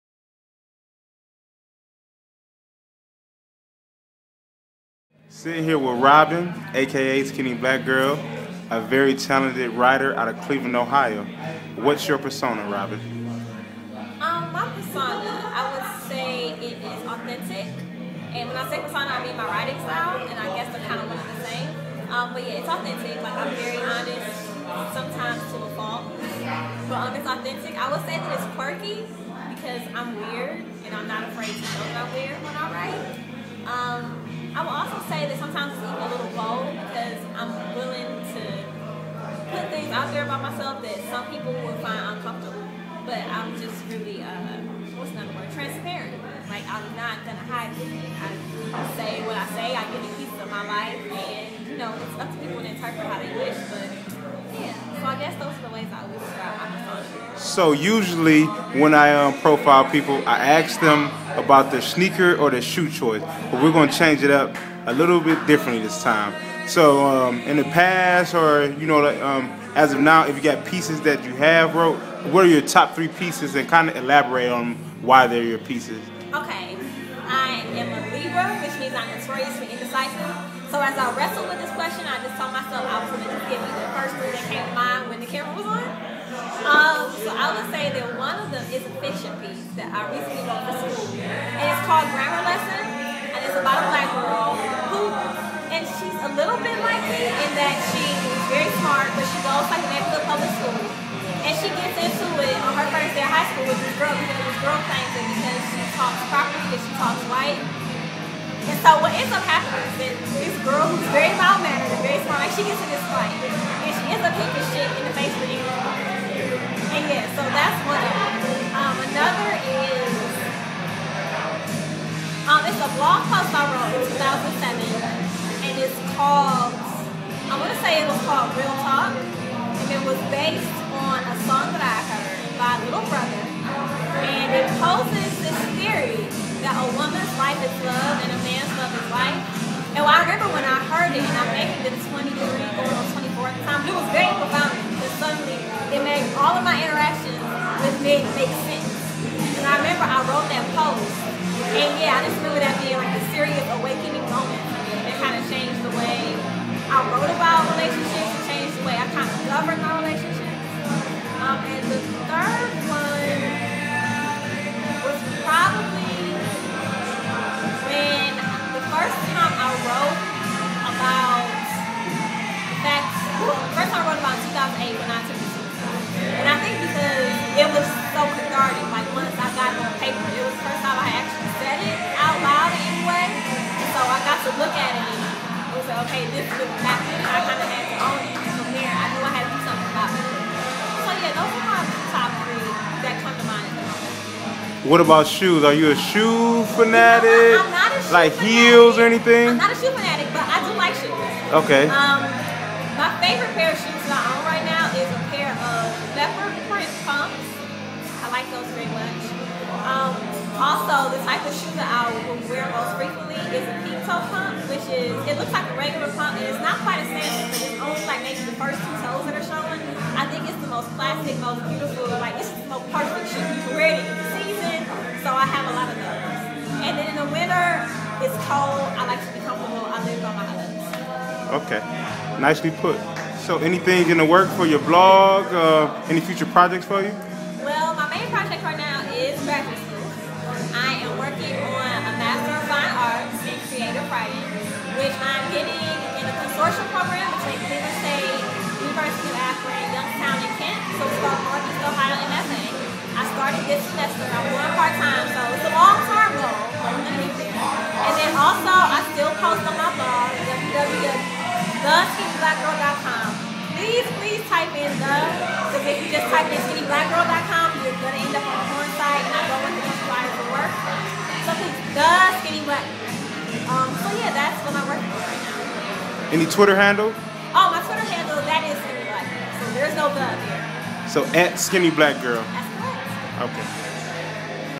YOP Profile: Skinny Black Girl
Your Own Persona teams up with writer Robin Thomas aka Skinny Black Girl.